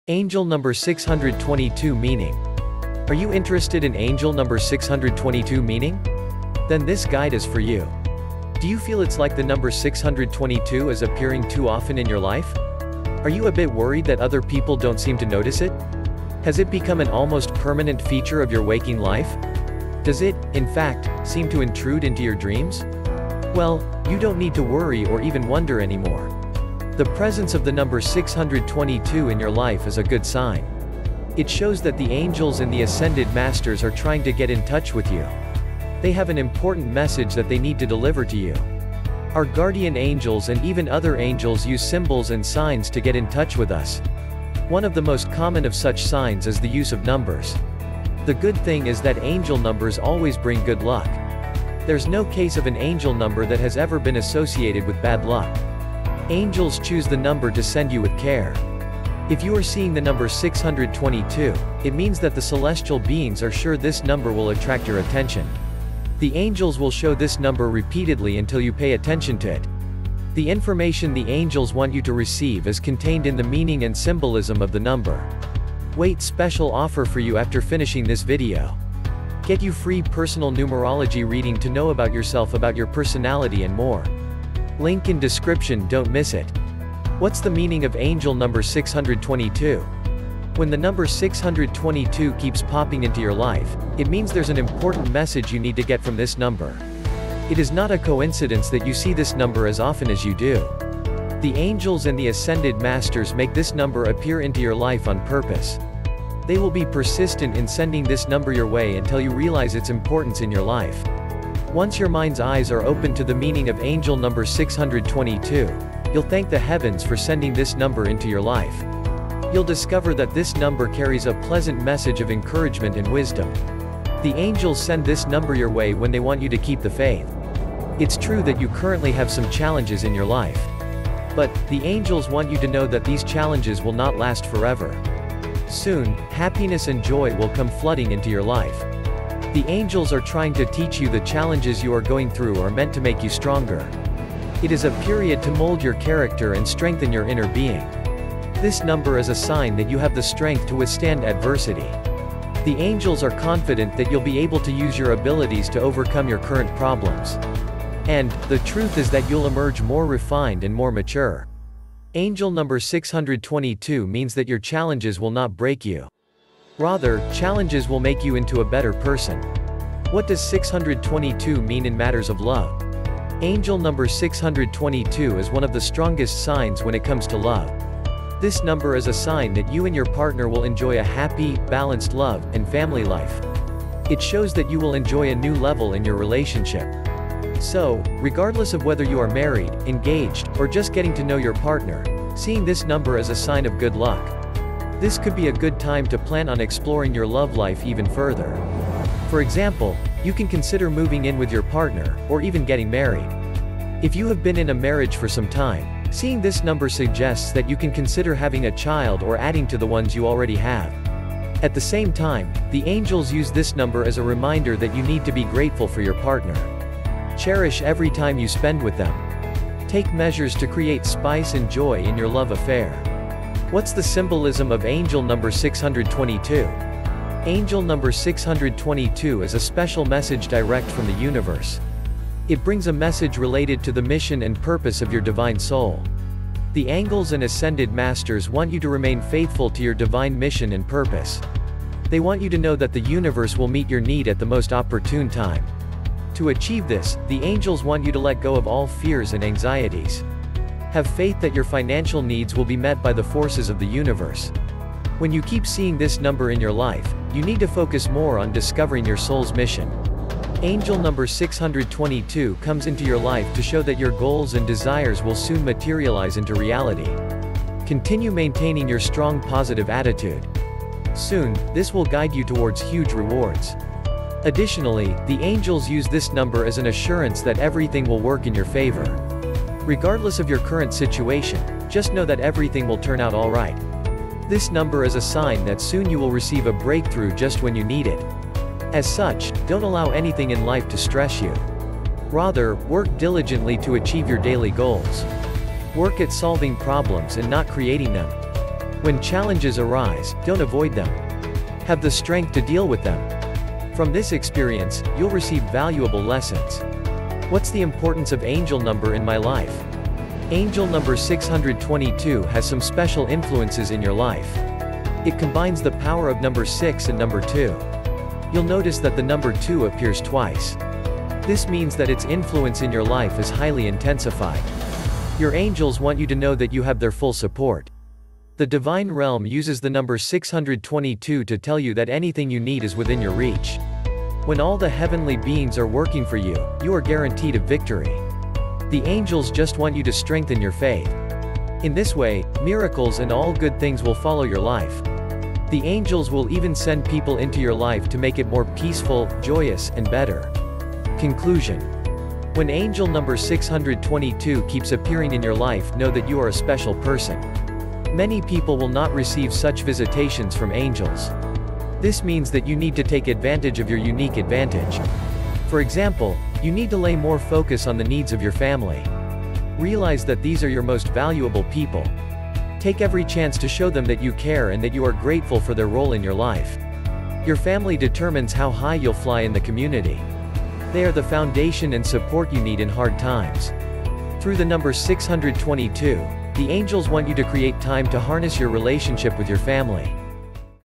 Angel Number 622 Meaning: Your Life Will Change Forever!
When the number 622 keeps popping into your life, it means there’s an important message you need to get from this number.

It is not a coincidence that you see this number as often as you do. The angels and the Ascended Masters make this number appear into your life on purpose.

They will be persistent in sending this number your way until you realize its importance in your life.

Once your mind’s eyes are open to the meaning of angel number 622, you’ll thank the heavens for sending this number into your life.

You’ll discover that this number carries a pleasant message of encouragement and wisdom.